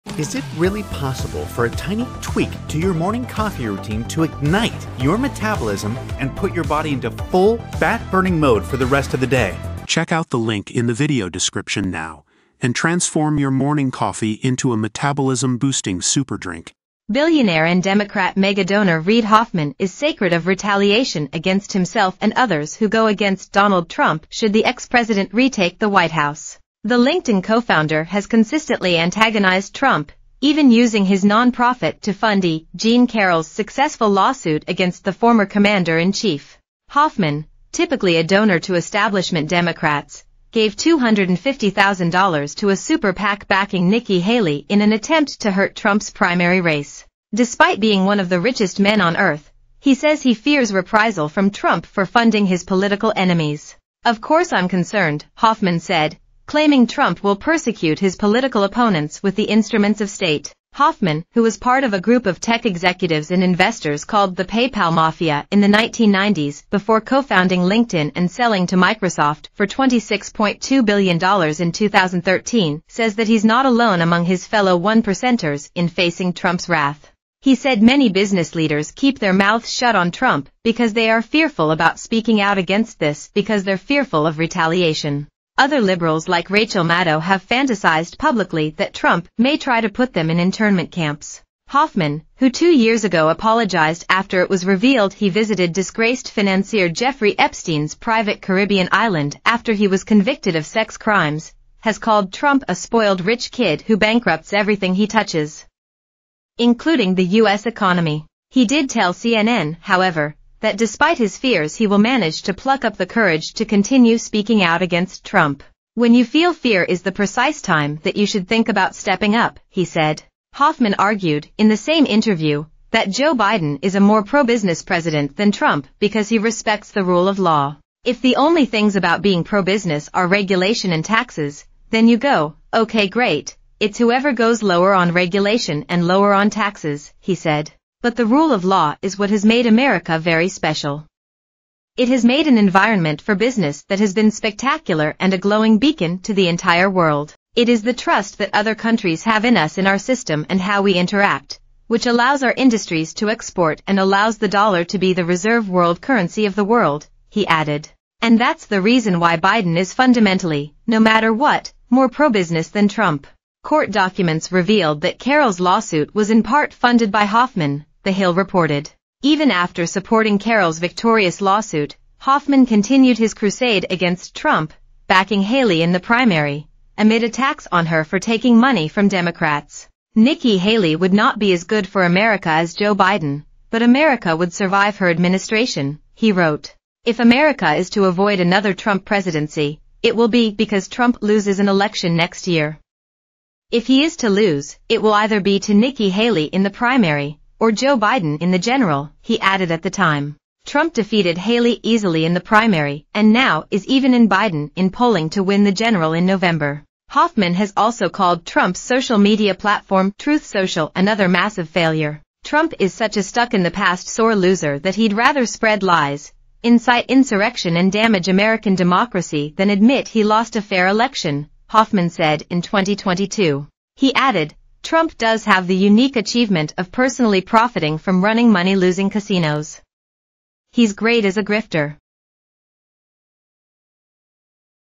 The billionaire LinkedIn co-founder who helped E. Jean Carroll sue Trump says he fears the former
The billionaire LinkedIn co-founder who helped E. Jean Carroll sue Trump says he fears the former president will be re-elected: 'Of course I'm worried'